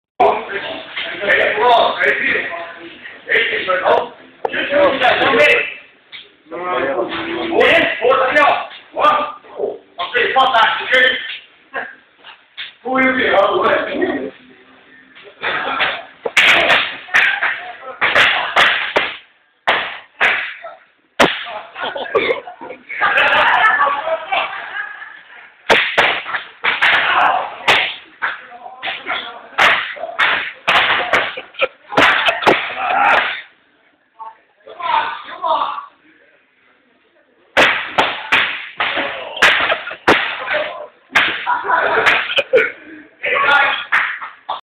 A Brazilian Jiu Jitsu Birthday Party
This is my birthday bashes at my jiu jitsu club :) was fun.